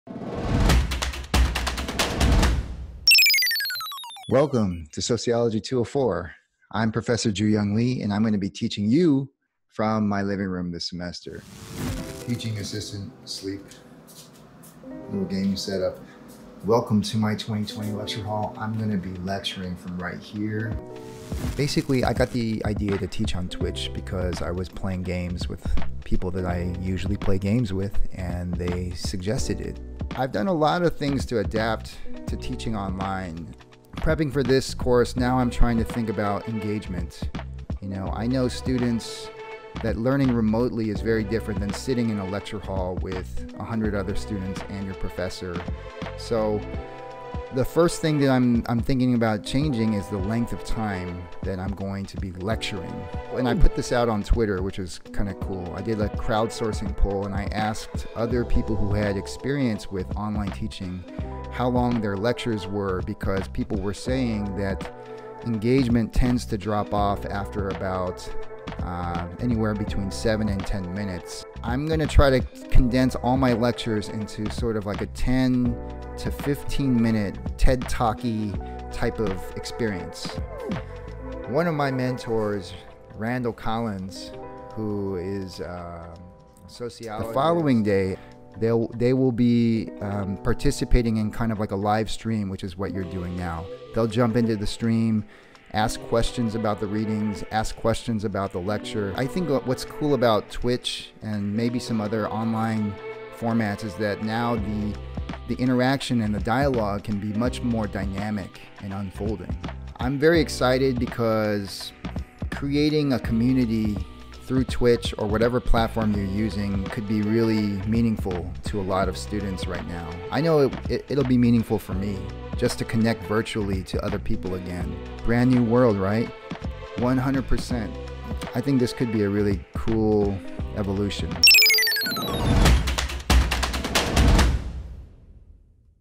Meet U of T prof Jooyoung Lee
Teaching on Twitch, U of T Professor Jooyoung Lee is changing the game on virtual classes. 10-15 minute TED Talk style lectures and livestreams promote interaction and dialogue. Would you join this class?